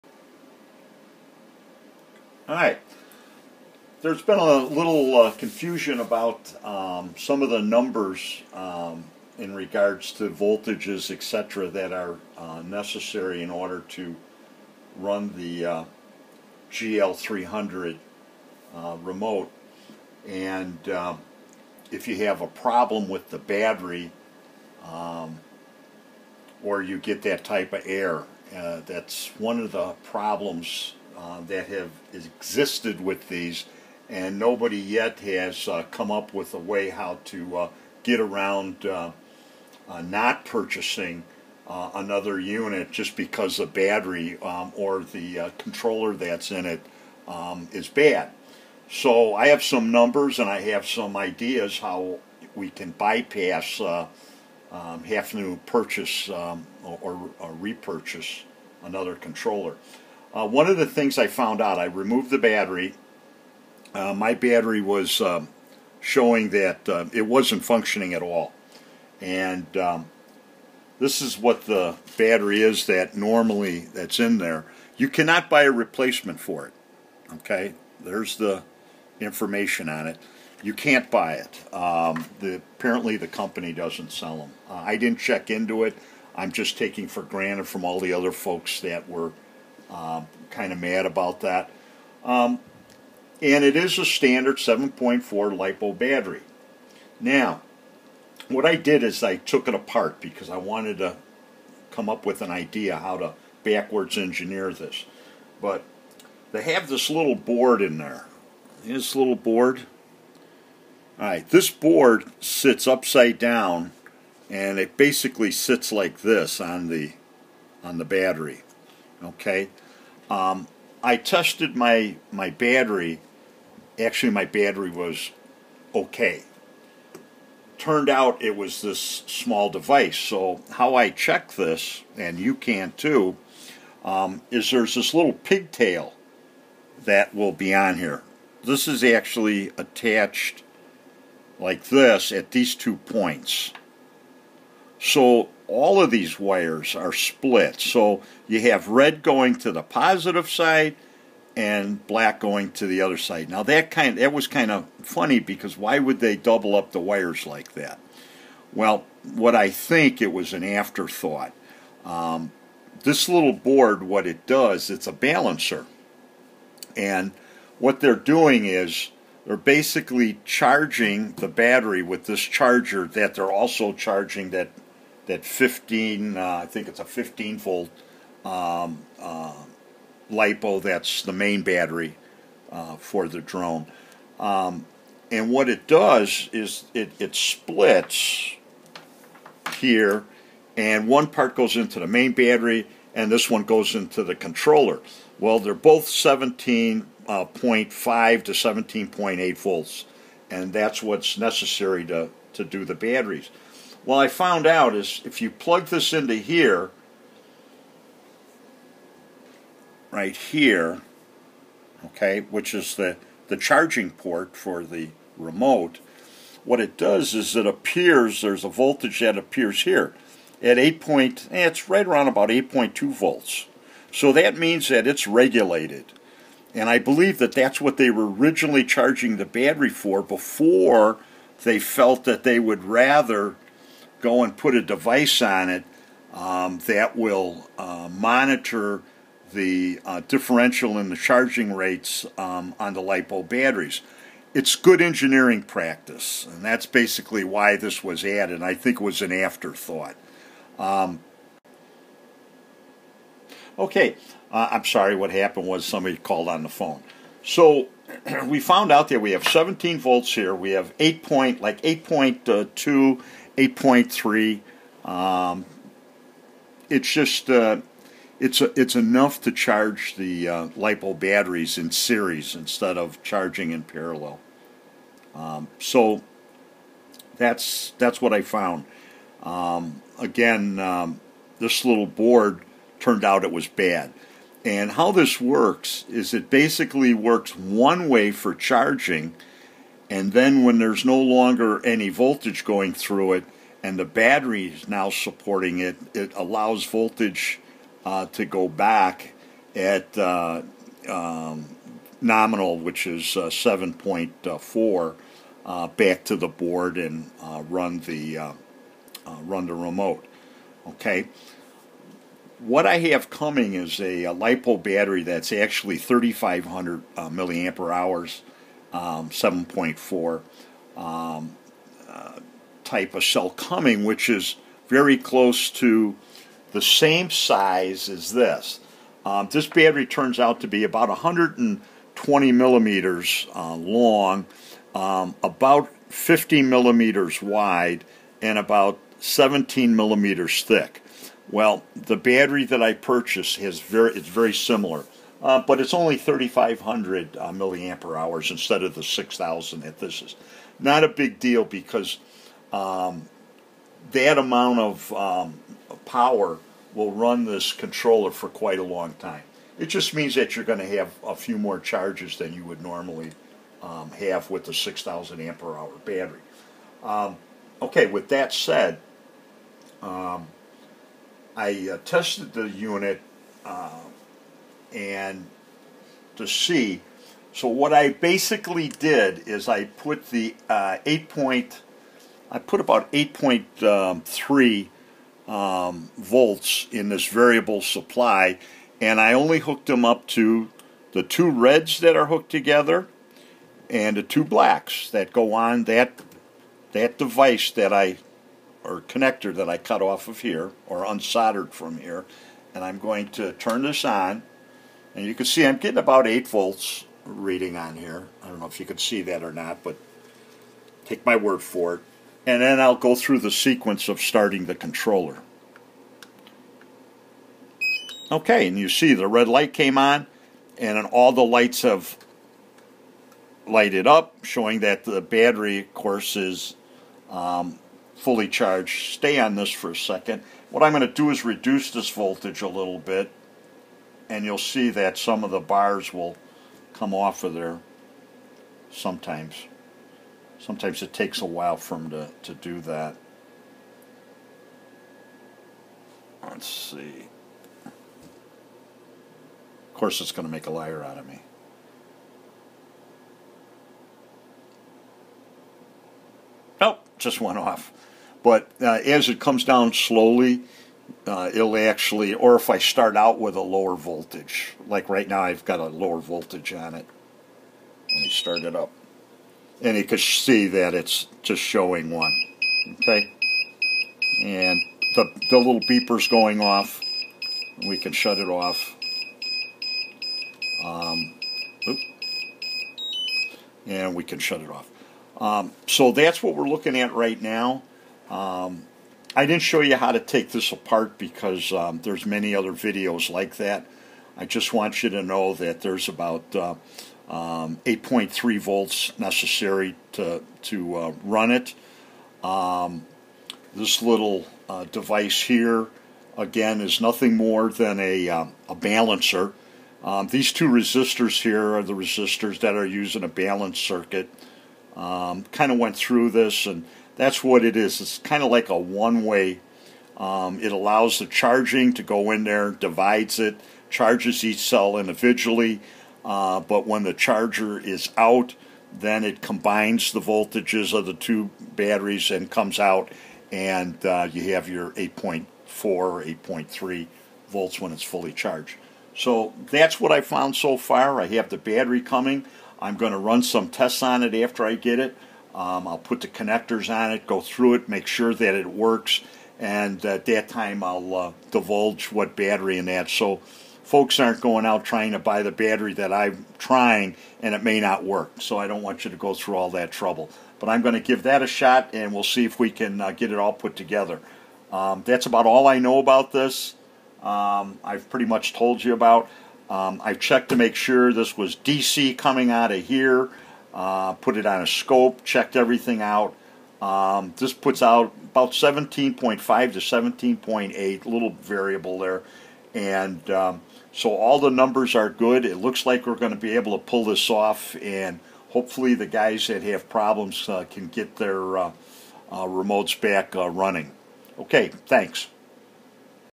Phanton 3 controller battery replacement GL300C Part 1
Looking and verifying battery and device voltages necessary to replace the controllers battery with the least amount of modifications to the unit.